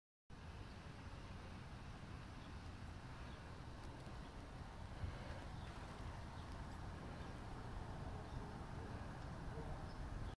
Pond worm eel like creatures
Strange life in my pond. small half or 25% the size of an earth worm,